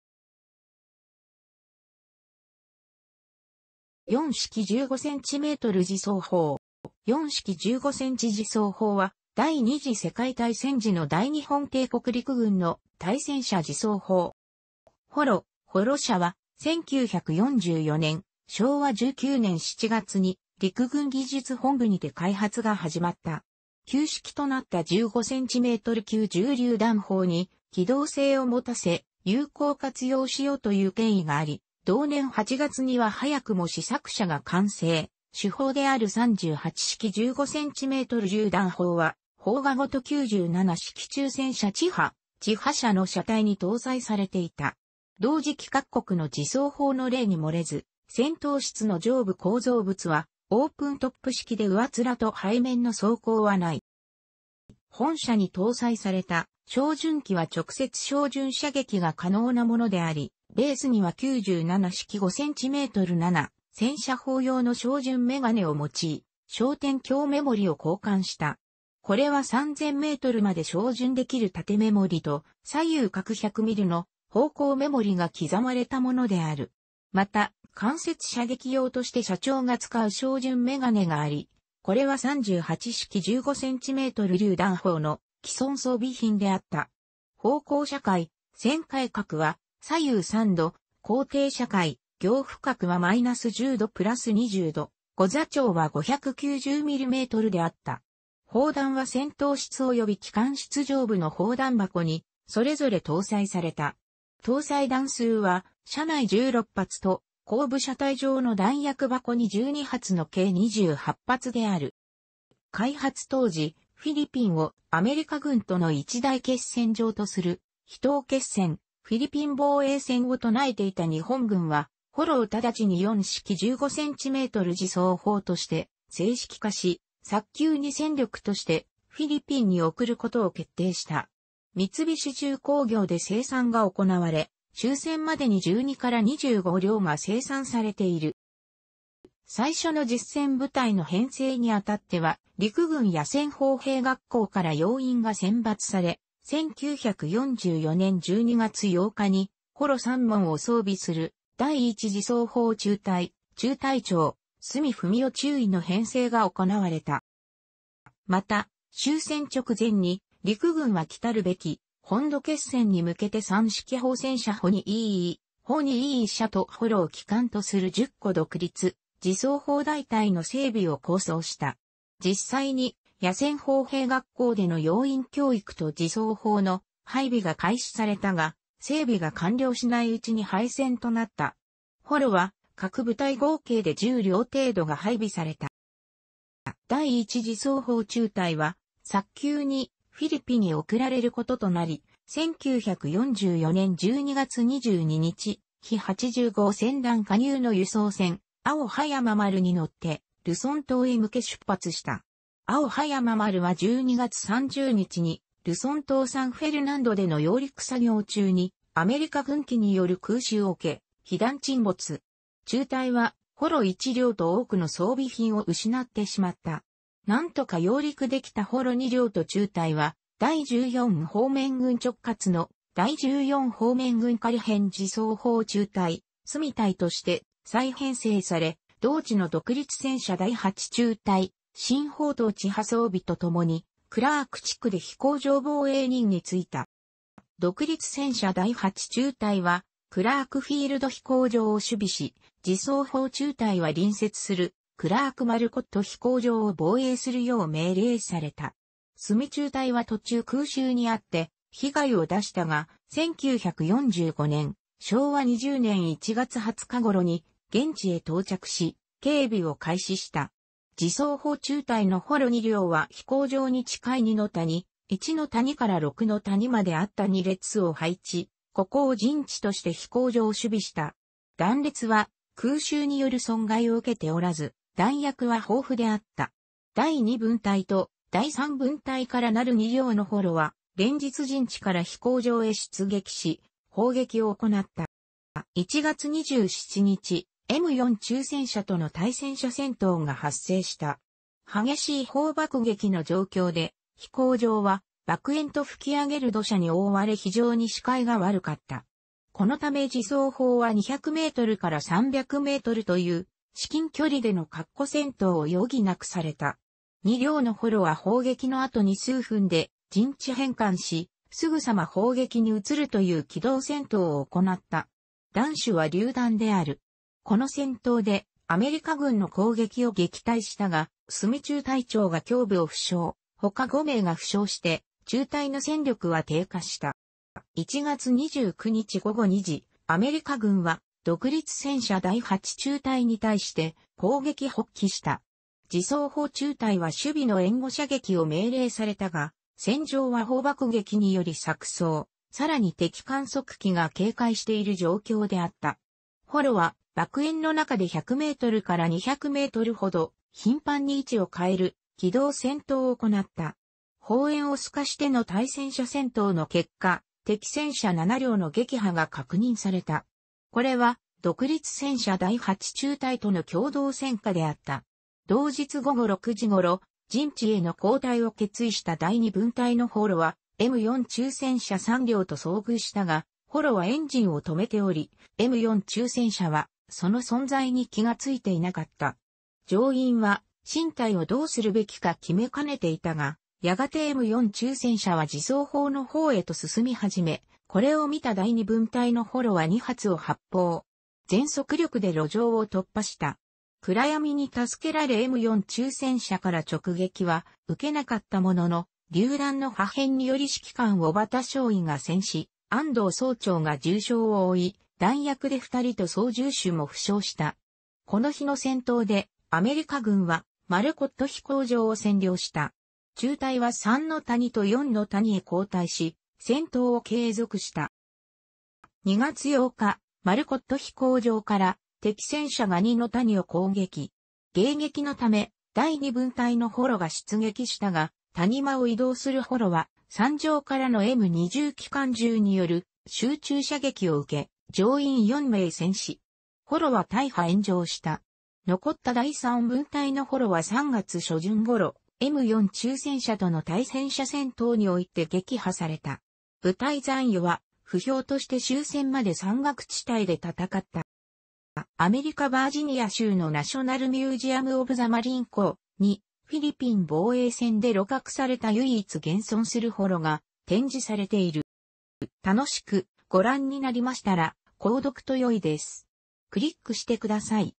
四式十五糎自走砲
四式十五糎自走砲（よんしきじゅうごせんちじそうほう）は、第二次世界大戦時の大日本帝国陸軍の対戦車自走砲。

ホロ（ホロ車）は、1944年（昭和19年）7月に陸軍技術本部にて開発が始まった。旧式となった15cm級重榴弾砲に機動性を持たせ、有効活用しようという経緯があり、 同年8月には早くも試作車が完成、主砲である三八式十五糎榴弾砲は砲架ごと九七式中戦車 チハ（チハ車）の車体に搭載されていた。同時期各国の自走砲の例に漏れず、戦闘室の上部構造物はオープントップ式で上面と背面の装甲は無い。

本車に搭載された照準器は直接照準射撃が可能なものであり、ベースには九七式五糎七戦車砲用の照準眼鏡を用い、焦点鏡目盛を交換した。これは3,000mまで照準できる縦目盛と、左右各100milの方向目盛が刻まれたものである。また、間接射撃用として車長が使う照準眼鏡があり、これは三八式十五糎榴弾砲の既存装備品であった。方向射界（旋回角）は左右3度、高低射界（仰俯角）は-10度～+20度、後座長は590mmであった。砲弾は戦闘室および機関室上部の砲弾箱にそれぞれ搭載された。搭載弾数は車内16発と後部車体上の弾薬箱に12発の計28発である。

開発当時、フィリピンをアメリカ軍との一大決戦場とする「比島決戦」（フィリピン防衛戦）を唱えていた日本軍は、ホロを直ちに四式十五糎自走砲として制式化し、早急に戦力としてフィリピンに送ることを決定した。三菱重工業で生産が行われ、終戦までに12-25輌が生産されている。

最初の実戦部隊の編成に当たっては陸軍野戦砲兵学校から要員が選抜され、1944年12月8日にホロ3門を装備する第1自走砲中隊（中隊長鷲見文男中尉）の編成が行われた。

また、終戦直前に陸軍は来るべき本土決戦に向けて三式砲戦車 ホニIII（ホニIII車）とホロを基幹とする10個独立自走砲大隊の整備を構想した。実際に野戦砲兵学校での要員教育と自走砲の配備が開始されたが、整備が完了しないうちに敗戦となった。ホロは各部隊合計で10輌程度が配備された。

第1自走砲中隊は早急にフィリピンへ送られることとなり、1944年12月22日、ヒ85船団加入の輸送船「青葉山丸」に乗ってルソン島へ向け出発した。「青葉山丸」は12月30日にルソン島サンフェルナンドでの揚陸作業中にアメリカ軍機による空襲を受け、被弾沈没。中隊はホロ1輌と多くの装備品を失ってしまった。なんとか揚陸できたホロ2輌と中隊は、第14方面軍直轄の第14方面軍仮編自走砲中隊（鷲見隊）として再編成され、同地の独立戦車第8中隊（新砲塔チハ装備）と共にクラーク地区で飛行場防衛任務に就いた。

独立戦車第8中隊はクラークフィールド飛行場を守備し、自走砲中隊は隣接するクラークマルコット飛行場を防衛するよう命令された。鷲見中隊は途中空襲にあって被害を出したが、1945年（昭和20年）1月20日頃に現地へ到着し、警備を開始した。自走砲中隊のホロ2輌は飛行場に近い二の谷（一の谷から六の谷まであった）に段列を配置、ここを陣地として飛行場を守備した。段列は空襲による損害を受けておらず、弾薬は豊富であった。第2分隊と第3分隊から成る2輌のホロは、連日陣地から飛行場へ出撃し砲撃を行った。

1月27日、M4中戦車との対戦車戦闘が発生した。激しい砲爆撃の状況で、飛行場は爆煙と吹き上げる土砂に覆われ非常に視界が悪かった。このため自走砲は200mから300mという至近距離での各個戦闘を余儀なくされた。2輌のホロは砲撃の後に数分で陣地変換し、すぐさま砲撃に移るという機動戦闘を行った。弾種は榴弾である。この戦闘でアメリカ軍の攻撃を撃退したが、鷲見中隊長が胸部を負傷、ほか5名が負傷して中隊の戦力は低下した。

1月29日午後2時、アメリカ軍は独立戦車第8中隊に対して攻撃発起した。自走砲中隊は守備の援護射撃を命令されたが、戦場は砲爆撃により錯綜、さらに敵観測機が警戒している状況であった。ホロは爆煙の中で100mから200mほど頻繁に位置を変える機動戦闘を行った。砲煙をすかしての対戦車戦闘の結果、敵戦車7両の撃破が確認された。これは独立戦車第8中隊との共同戦果であった。同日午後6時頃、陣地への後退を決意した第2分隊のホロはM4中戦車3両と遭遇したが、ホロはエンジンを止めており、M4中戦車はその存在に気が付いていなかった。乗員は進退をどうするべきか決めかねていたが、やがてM4中戦車は自走砲の方へと進みはじめ、これを見た第2...